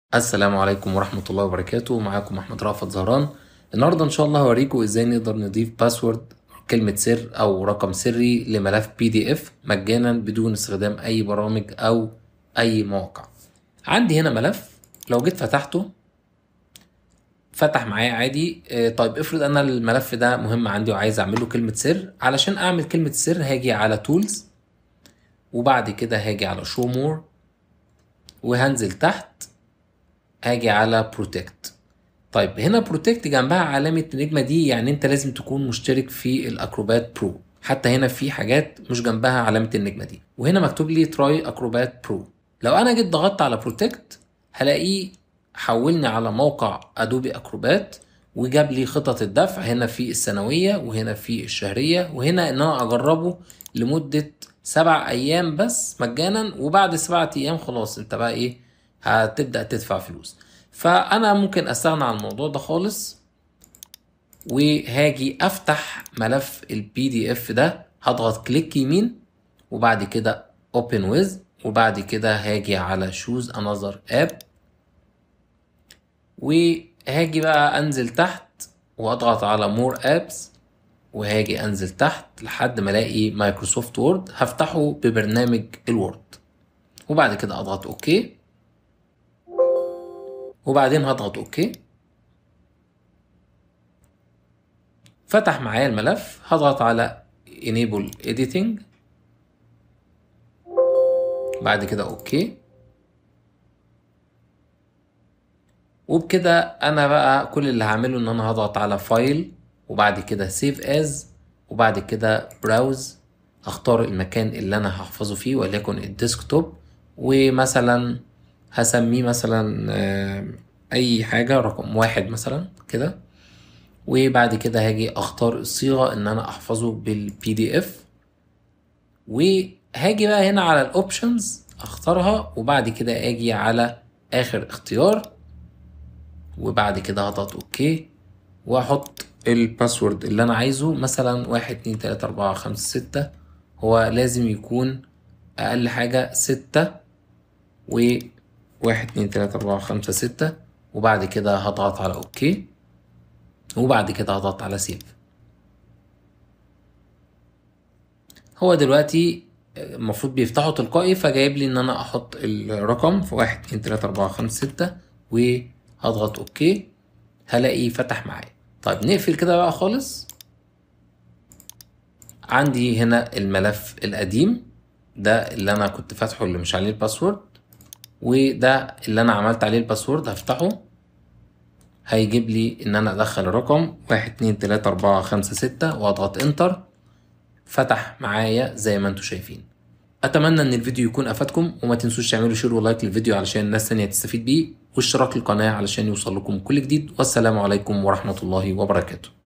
اضافة كلمة سر لملف PDF
شرح طريقة اضافة كلمة السر لحماية ملف PDF بطريقة مجانية بدون الاشتراك او استخدام اي برامج او مواقع